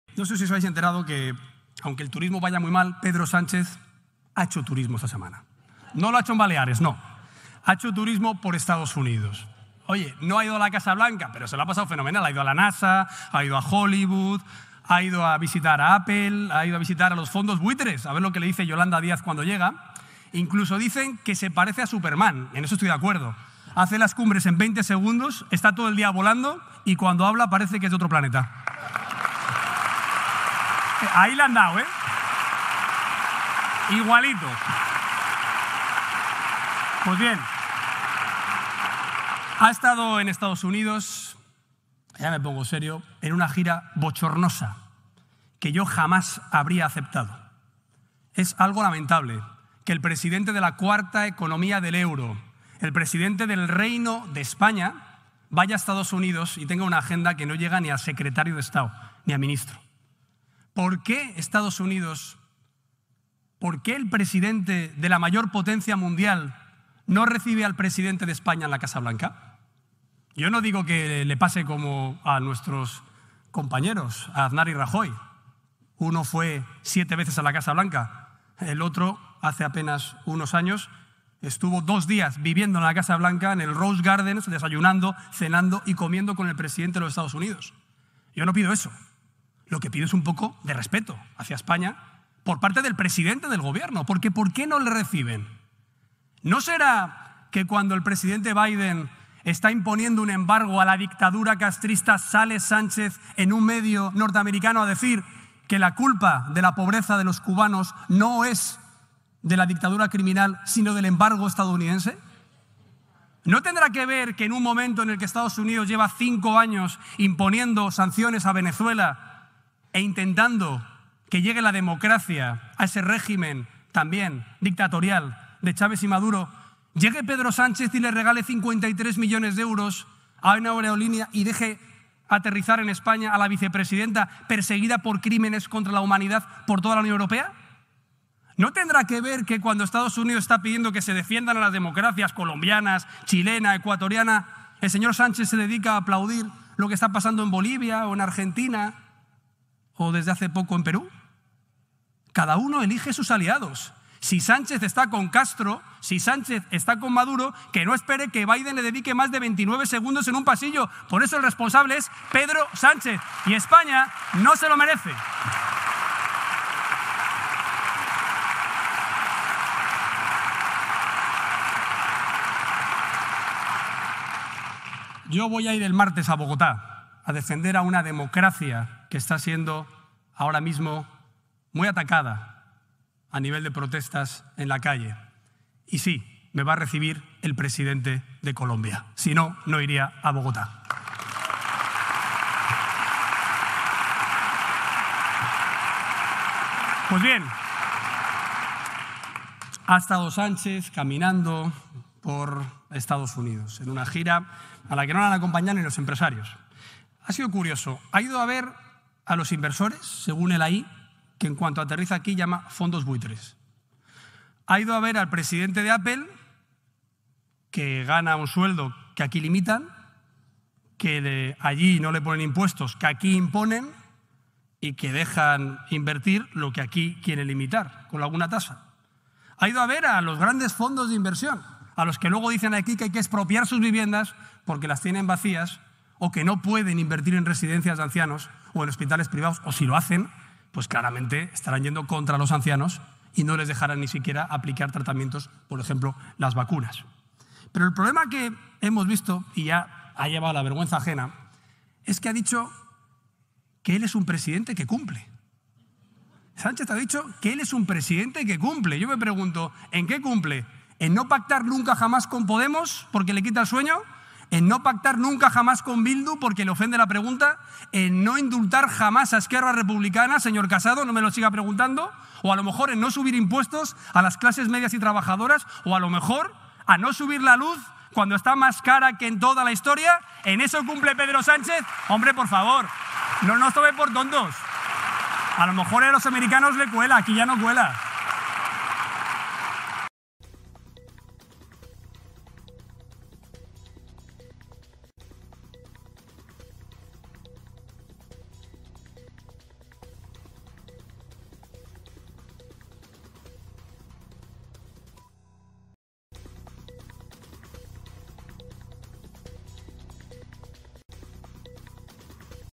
💥¡MAZAZO A SÁNCHEZ!💥DESVELAN ¡TODA LA VERDAD! de por qué NINGÚN DIRIGENTE le ha RECIBIDO en EE.UU.
Pablo Casado. Líder del PP.